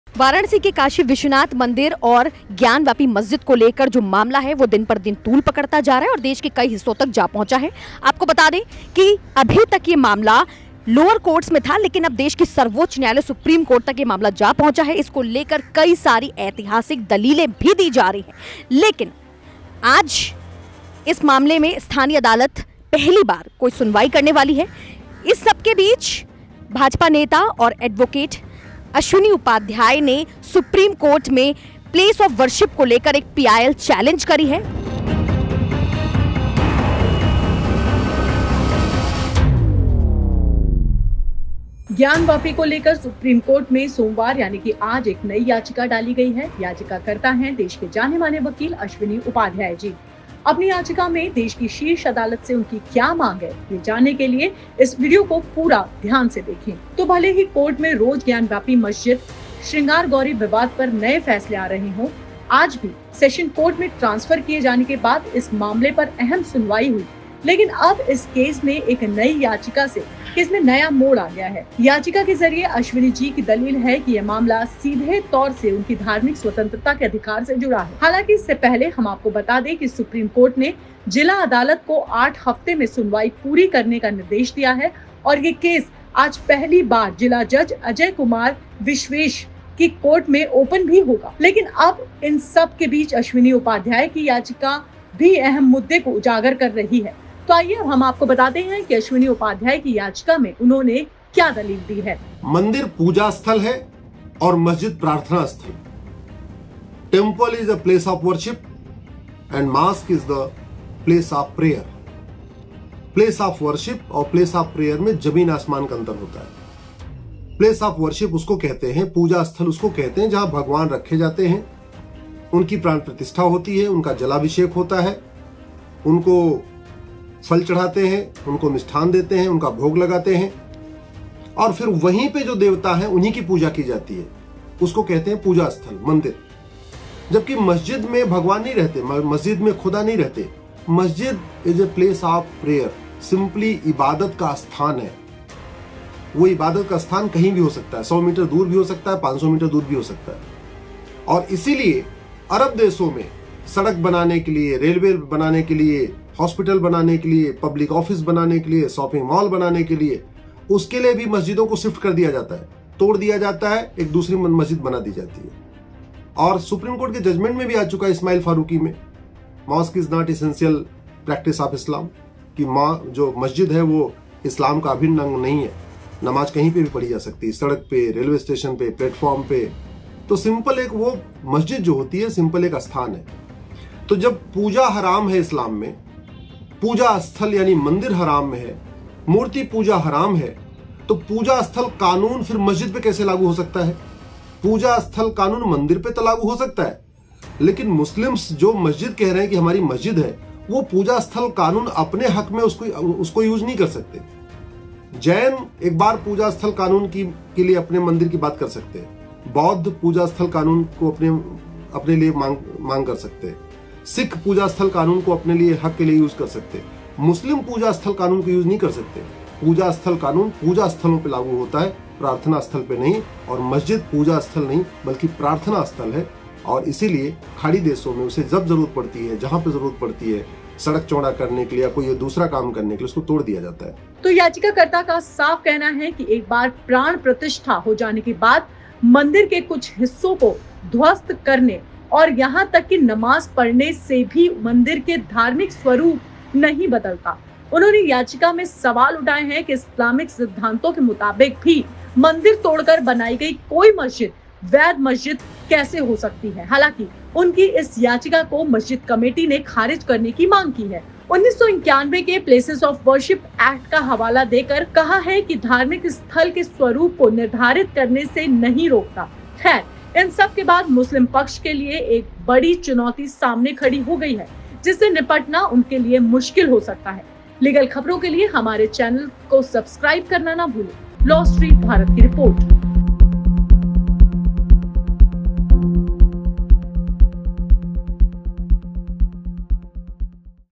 Why Places of Worship Act, 1991 Does Not Apply to Gyanvapi Matter?
जानिये क्यों नही लागू होता ज्ञानवापी पर 1991 का प्लेसेस ऑफ वर्शिप कानून || Why Places of Worship Act, 1991 Does Not Apply to Gyanvapi Matter?

ज्ञानवापी पर एडवोकेट अश्विनी उपाध्याय की याचिका में दी गई दलीलों के बाद मुस्लिम पक्ष परेशान है। जिस कानूनी तथ्यों के आधार पर याचिका डाली गई है उसके बाद आज देश में इस याचिका को लेकर काफी चर्चा हो रही है क्या है याचिका में दलील देखिये रिपोर्ट।